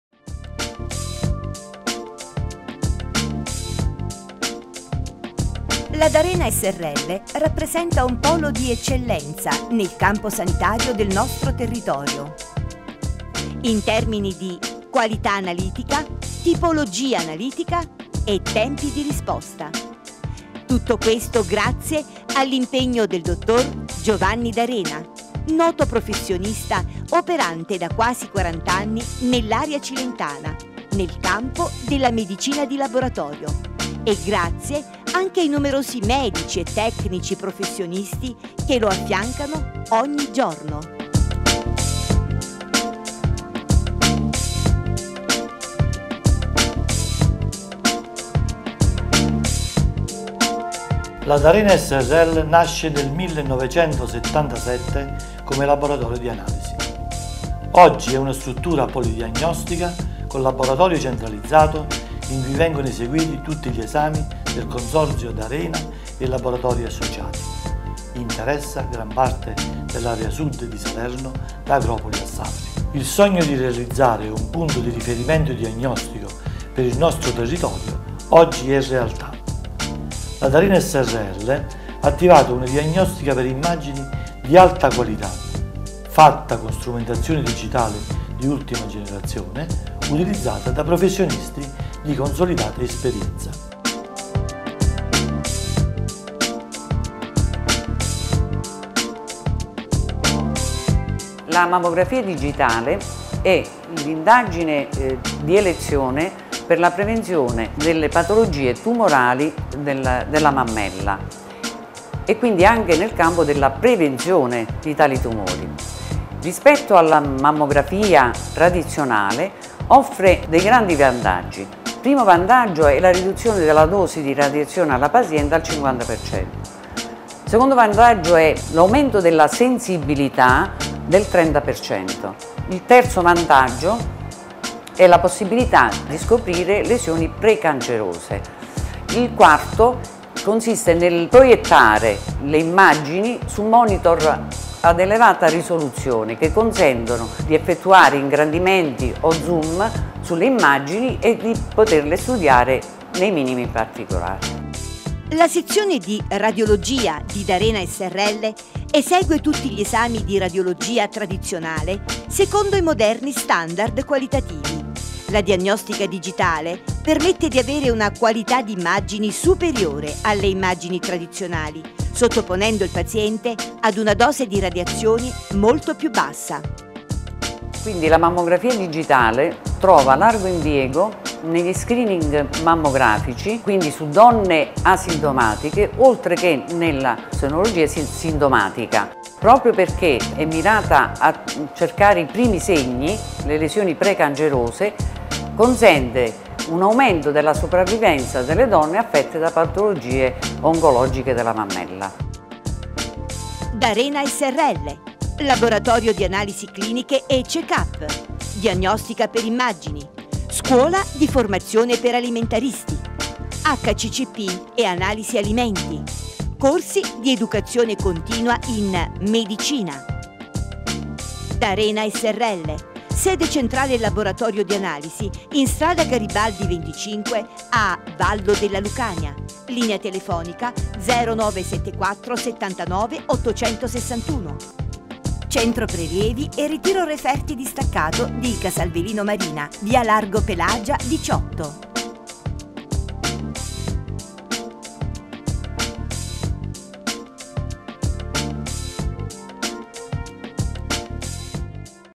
D'ARENA SRL - Analisi cliniche e Diagnostica per immagini in provincia di Salerno
Laboratorio D'Arena: analisi cliniche e diagnostica per immagini. Analisi del sangue, radiografie, tac, ecografia, ecodoppler, colonscopia. Certificazioni HACCP, Corsi di formazione per alimentaristi nella provincia di Salerno, Cilento, Vallo della Lucania.